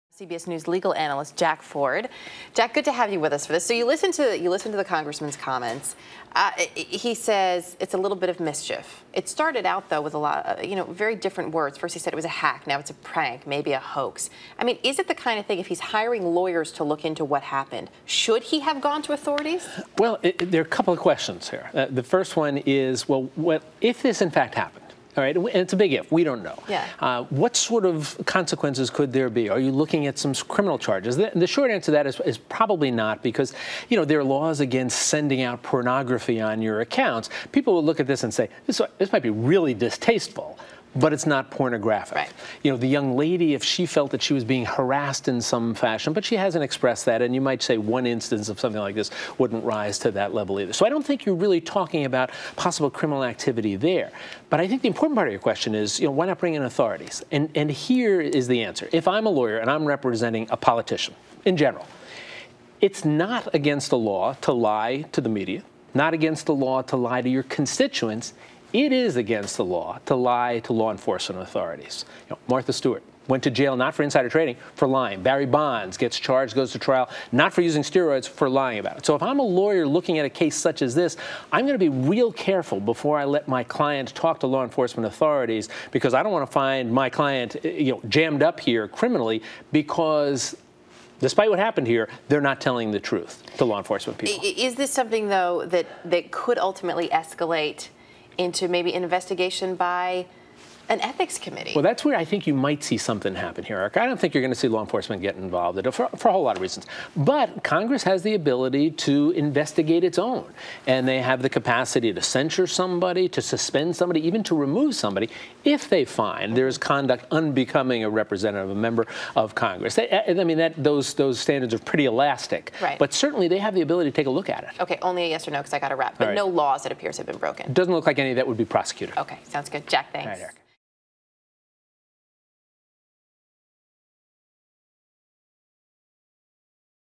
Legal implications of supposed Weiner photo
CBS News legal analyst Jack Ford speaks to Erica Hill about the release of a lewd photo on New York Rep. Anthony Weiner's twitter account and the legal implications of the photo.